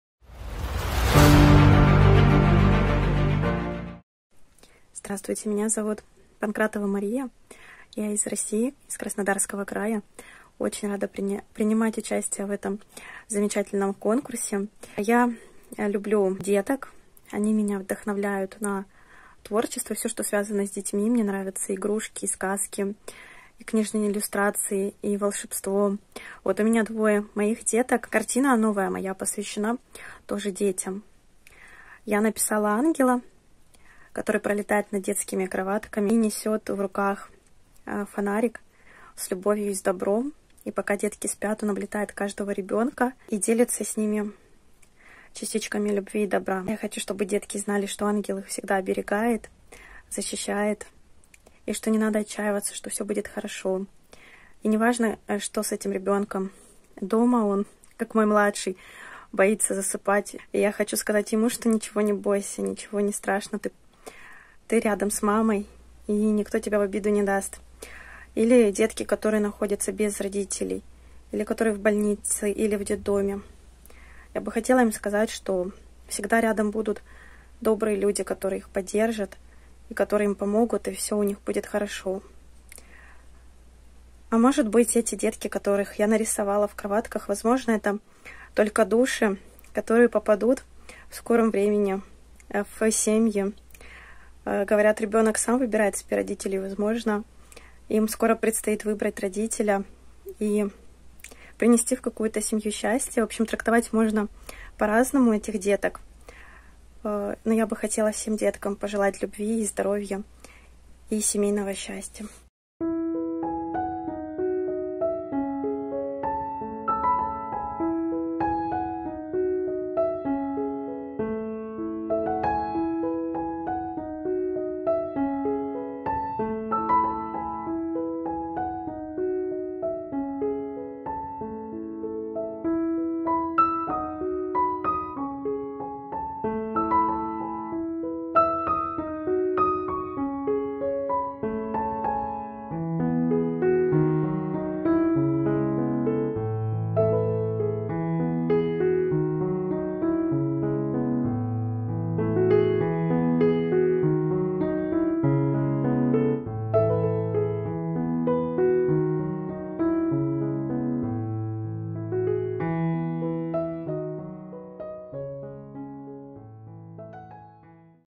Golden Time Talent | GRAND FINAL ART2 | Maria Pankratova | PAINTING
Имя участника или коллектив: Maria Pankratova

Страна: Russia

Город: станица Вознесенская

Название конкурсной работы: Под крылом ангела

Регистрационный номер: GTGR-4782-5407

Номинация: Живопись

Участие в других конкурсах и фестивалях / творческие достижения: Член Союза русских художников, победитель конкурса Golden Time 2021 года